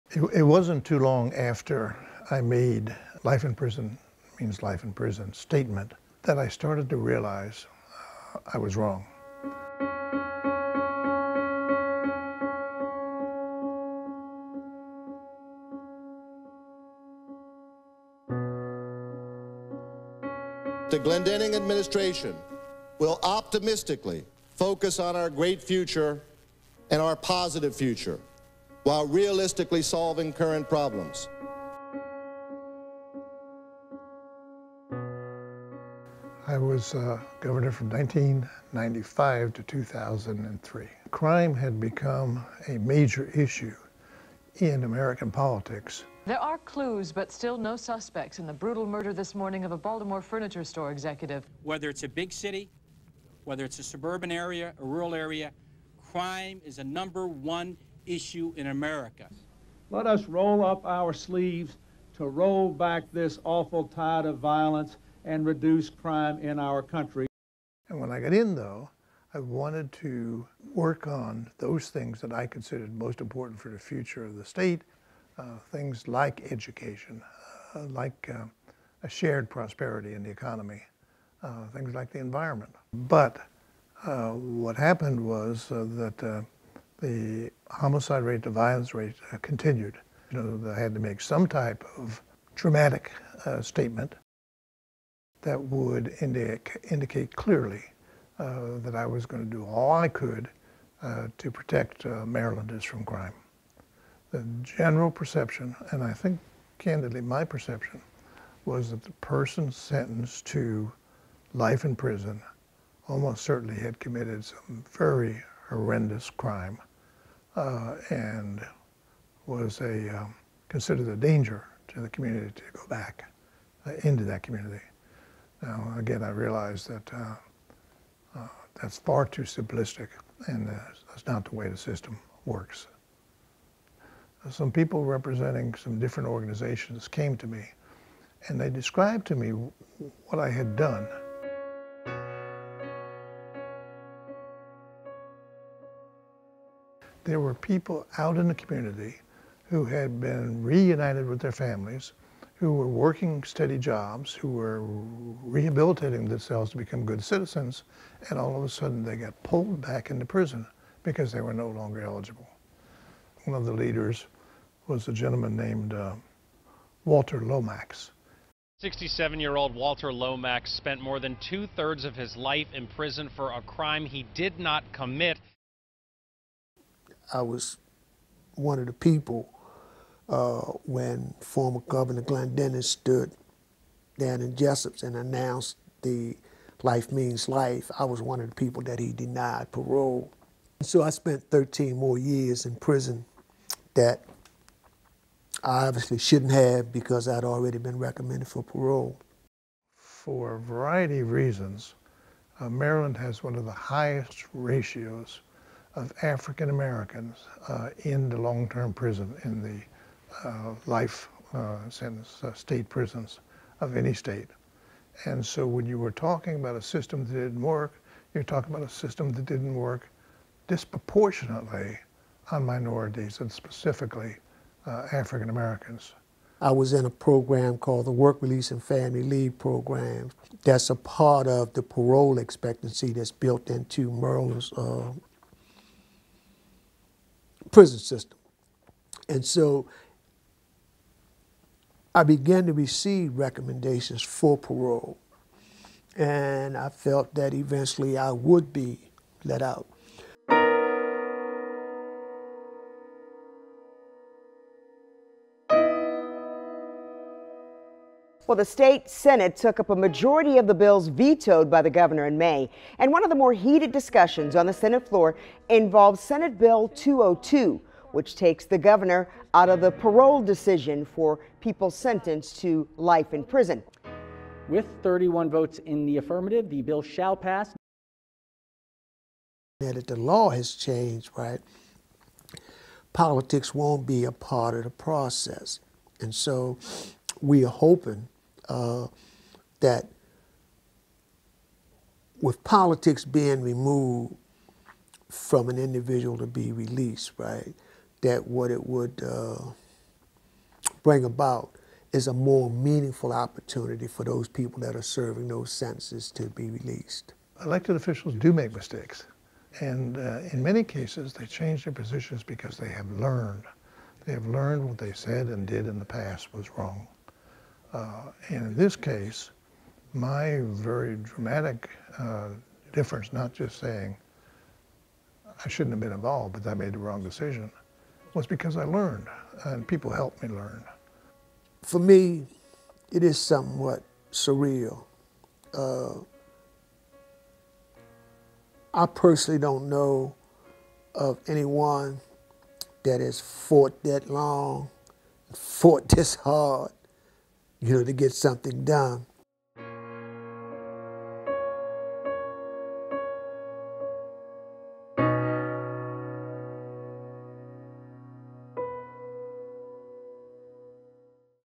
'I was wrong’: A governor’s choice cost hundreds a chance at freedom
Parris Glendening (D), a former Maryland governor, declared in 1995 that inmates serving a life sentence would be denied any chance of parole. For decades, after Glendening's statement, no one serving a life sentence was granted parole.

The policy, maintained by governors from both parties, left hundreds of prisoners with parole-eligible sentences — the vast majority of them Black men — to grow old and die in prison. Today, Glendening talks about his regrets and we hear from exoneree Walter Lomax, a man who was denied parole.